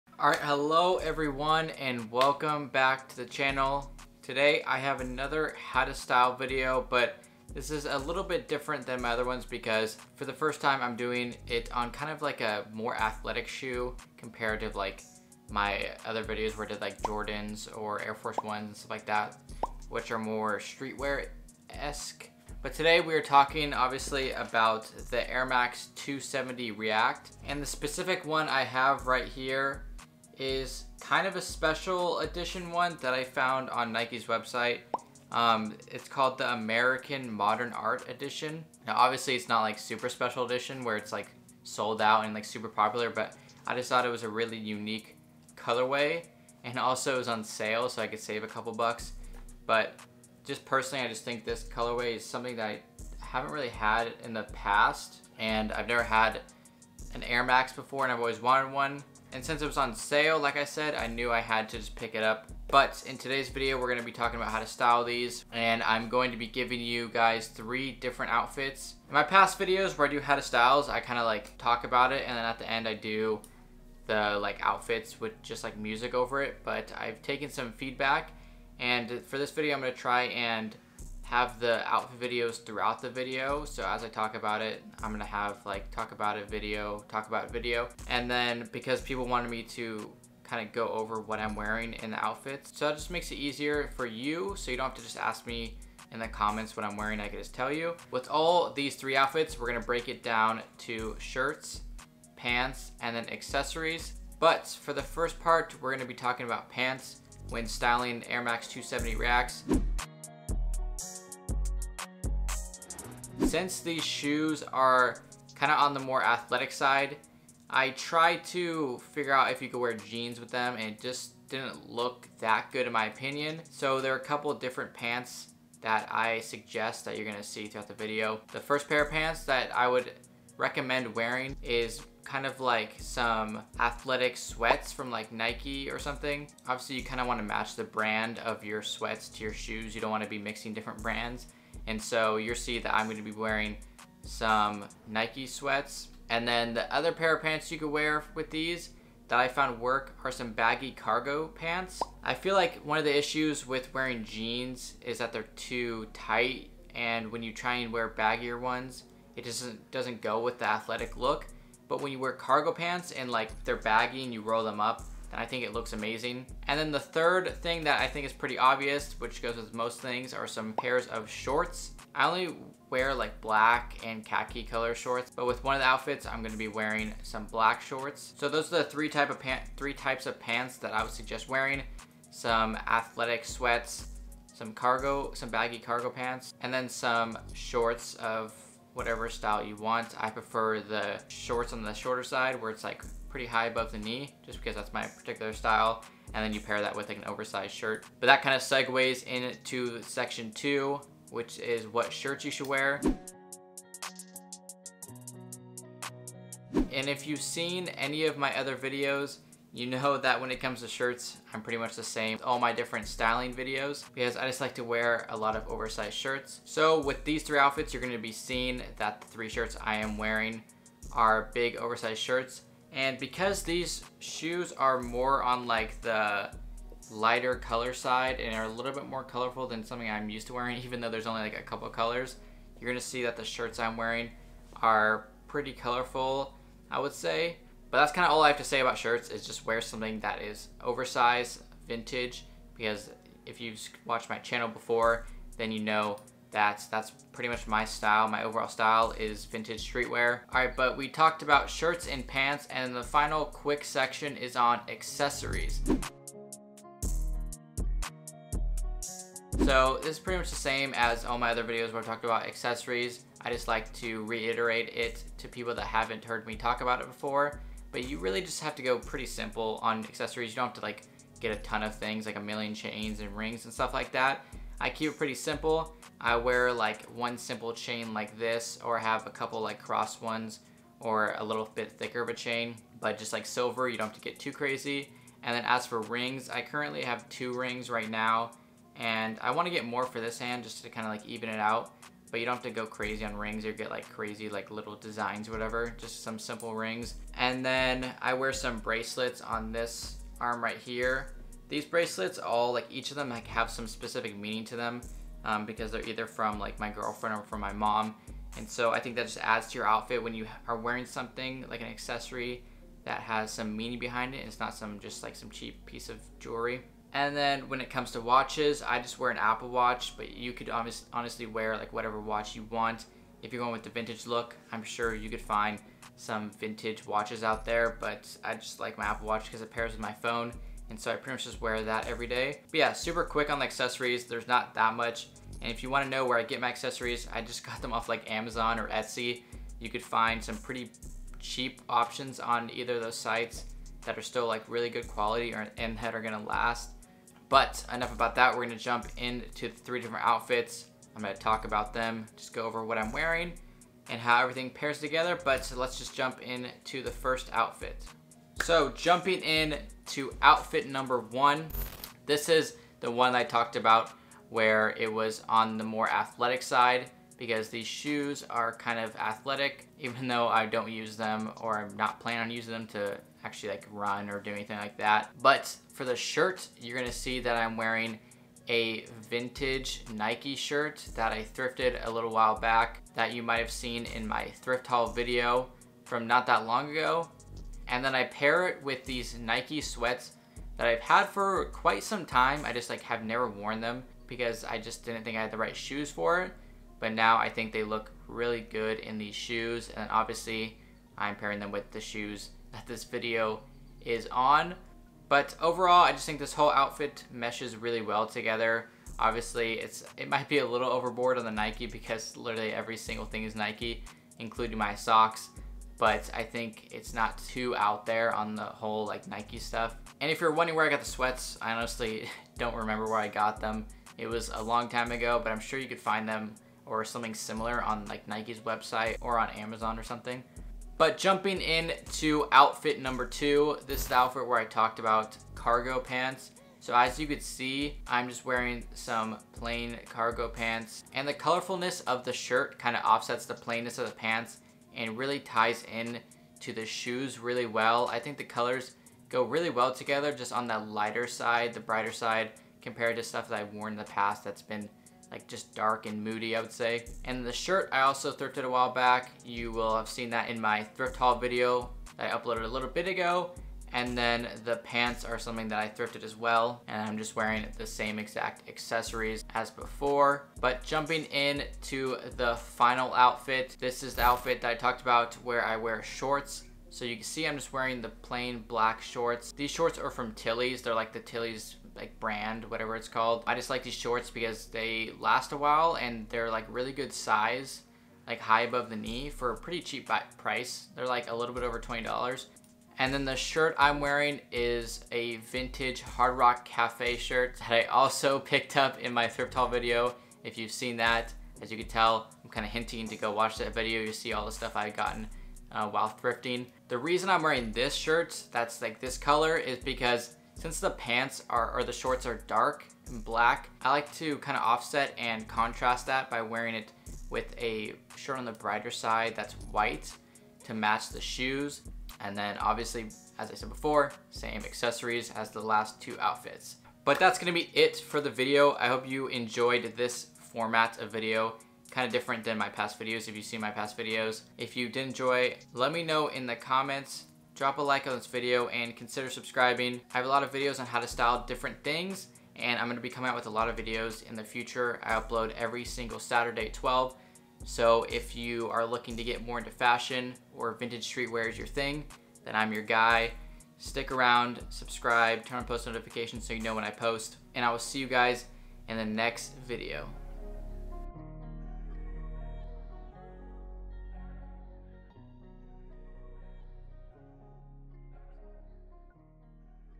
HOW TO STYLE: NIKE AIR MAX 270 REACT (American Modern Art) | Air Max 270 Lookbook | Air Max Outfits
How to style the Nike Air Max 270 react (American Modern Art), lookbook style, showing 3 Air Max 270 outfits. Hope you enjoy!

Background Music!